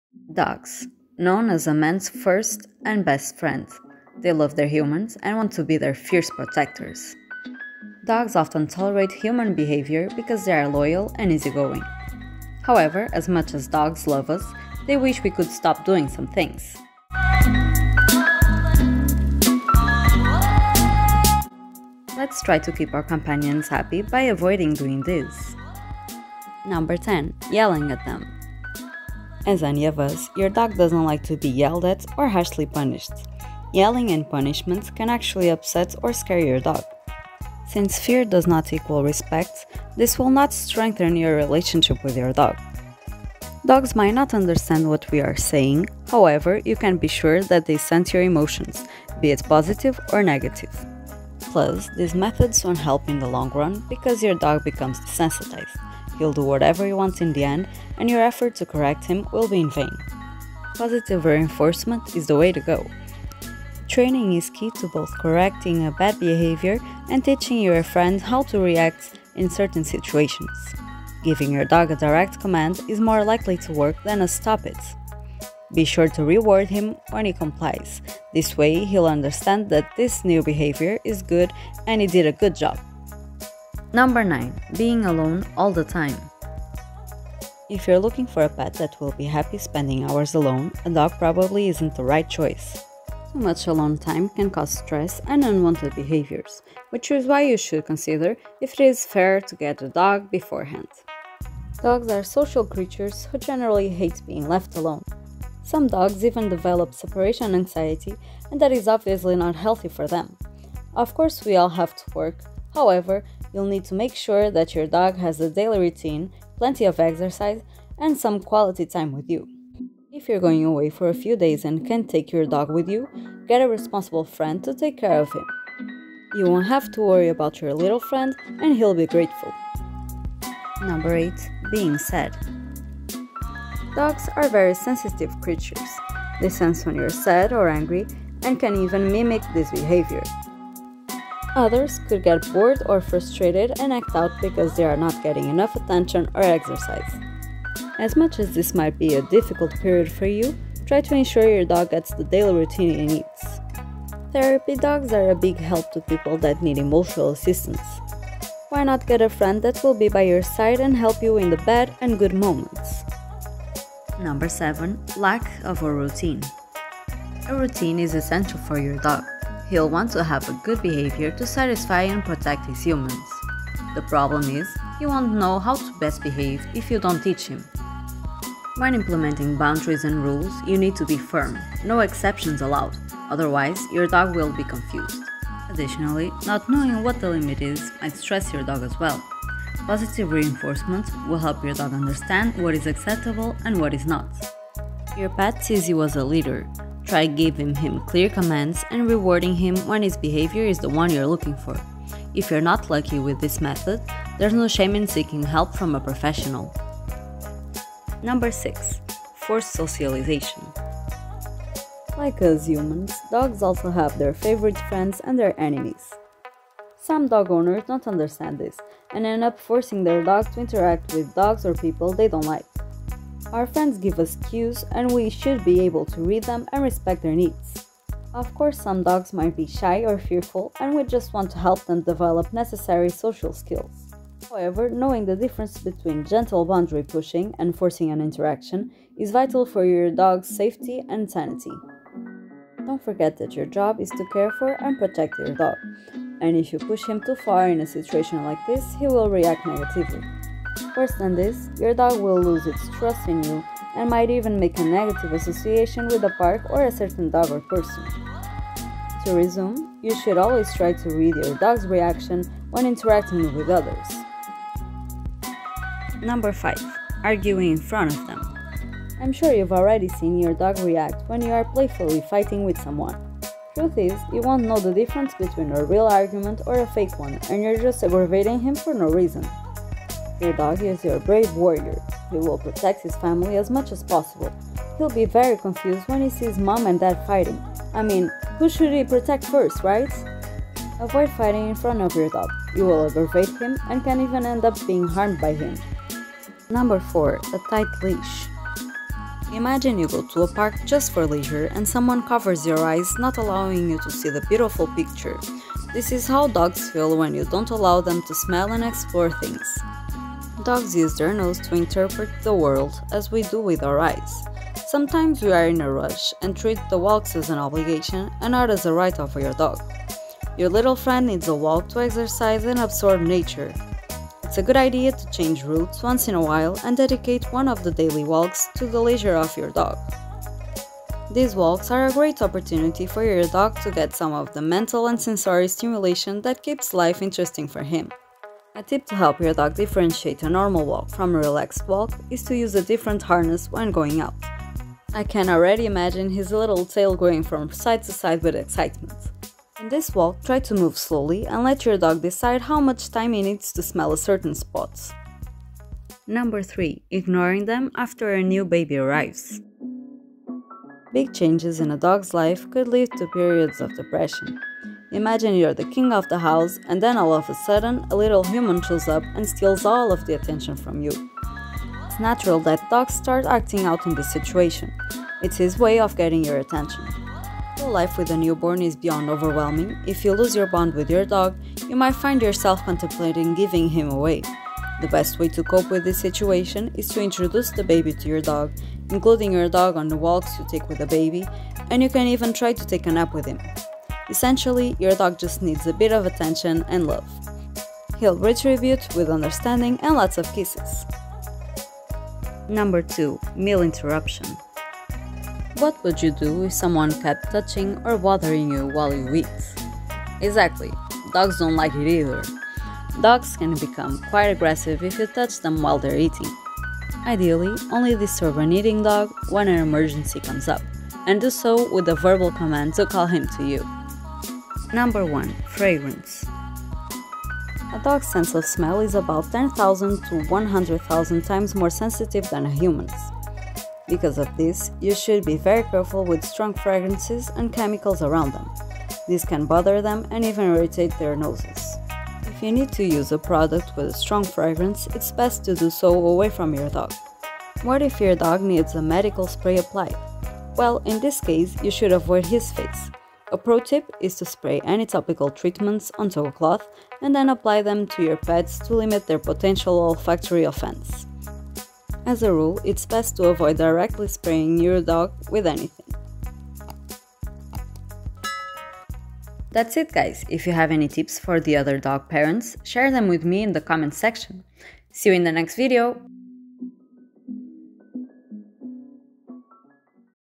What Does Your Dog Hate? - Human Behaviors Dogs Hate Part 1
Our dogs are part of our family, and we do everything we can to make them comfortable and healthy.
As much as we try to be the best dog owners, sometimes we can make some mistakes.
Check out this video that will show you what you could be doing wrong!

0:00 Intro
0:26 Yelling At Them
1:23 Being Alone
2:10 Being Sad
2:44 Lack Of A Routine
3:28 Forced Socialization
4:26 Arguing In Front Of Them
4:56 A Tight Leash
5:56 Being Ignored After a Baby Arrives
6:42 Meal Interruption
7:04 Fragrances

CREDITS